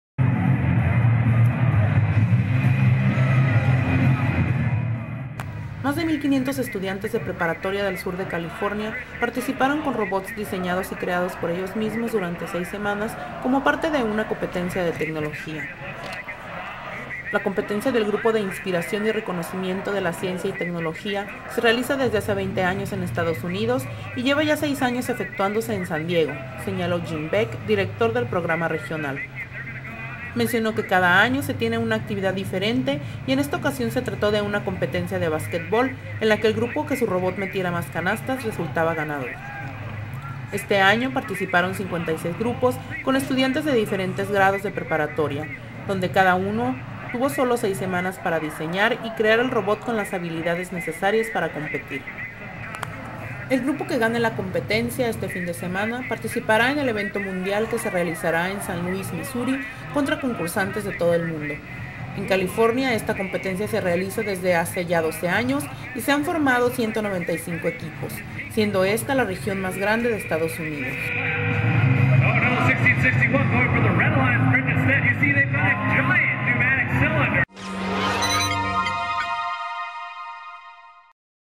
Compiten más de mil 500 estudiantes con robots
Más de mil 500 estudiantes de preparatoria del Sur de California, participaron con robots diseñados y creados por ellos mismos durante seis semanas, como parte de una competencia de tecnología. Video: Ana Gómez Salcido